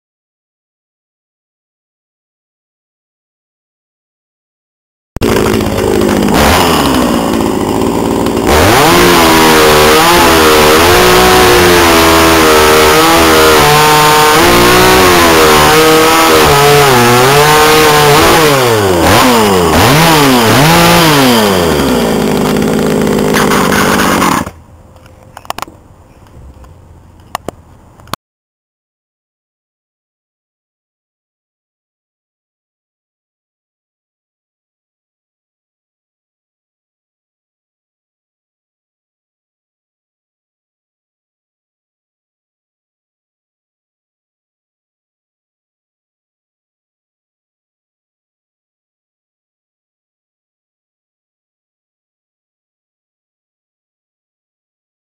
Film.wmv
Hier Spiele Ich ein bisschen mit meiner Motorsäge
habe Samstag bei meiner besten Freundin Brennholz gemacht.